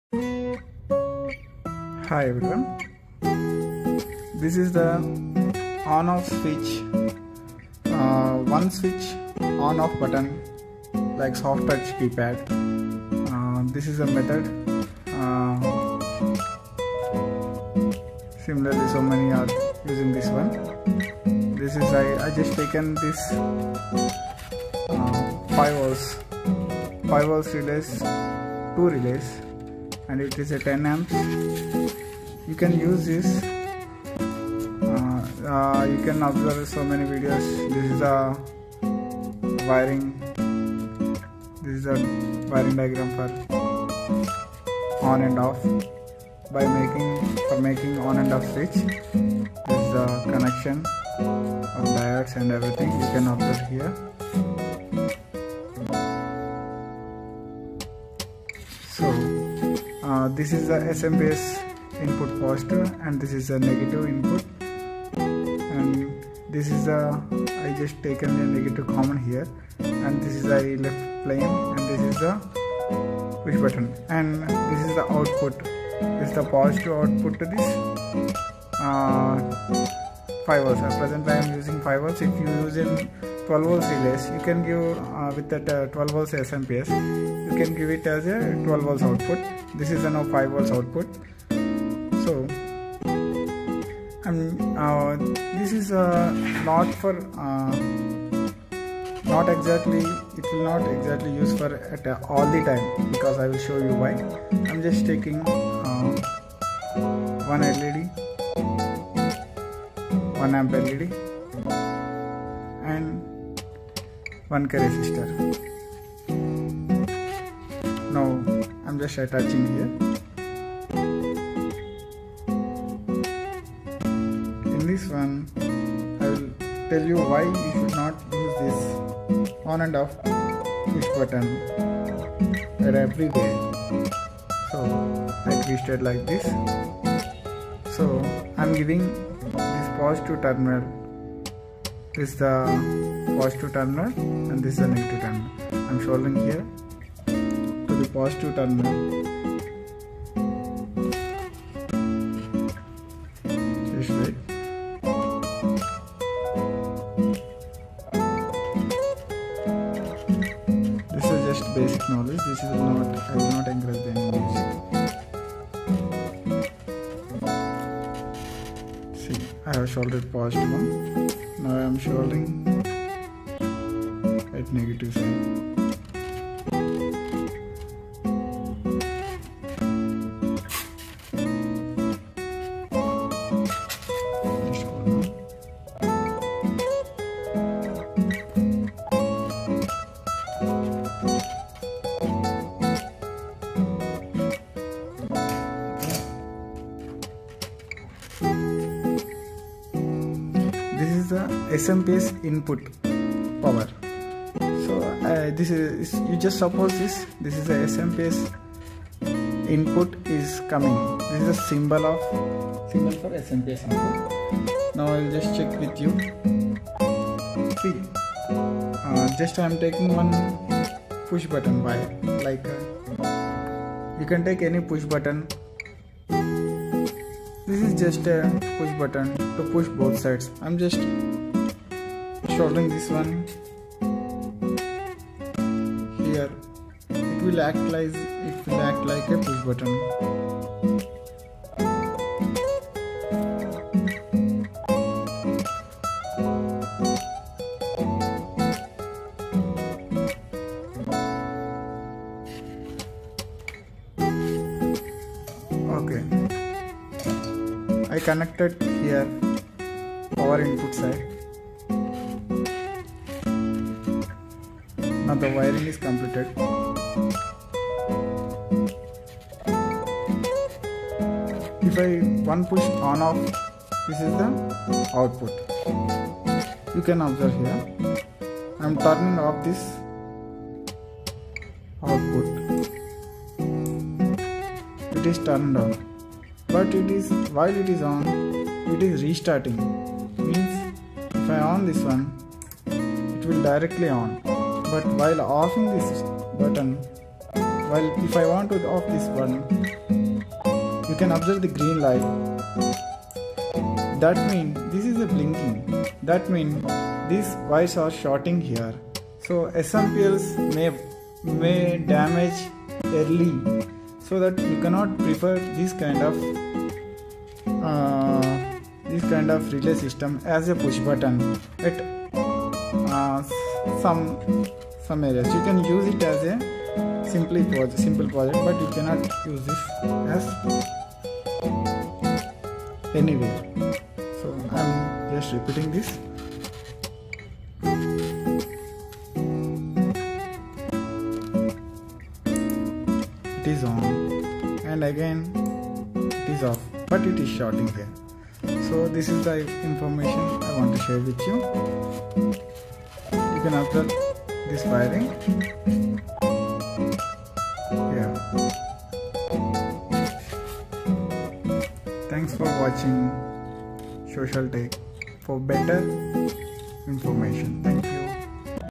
One Touch to on and off button / one touch on and off switch circuit /one touch on off
On off button with two relays, on and off button making , one button to on and one button to off , threat in two relays on and off